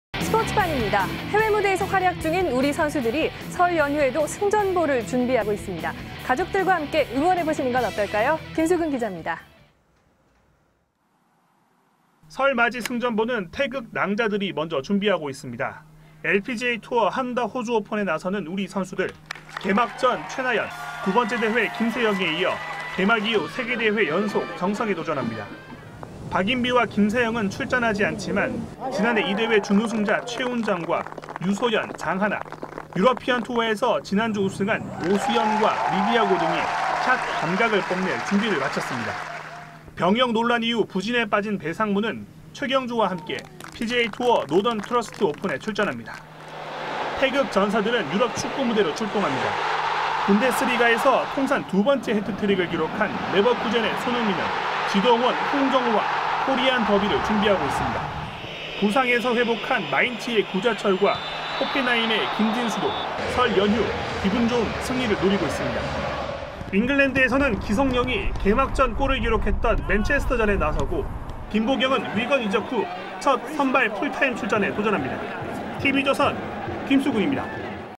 설맞이 승전보, 누가 먼저?
[앵커]
해외 무대에서 활약 중인 우리 선수들이 설 연휴에도 승전보를 준비하고 있습니다. 가족들과 함께 응원해보는 것은 어떨까요?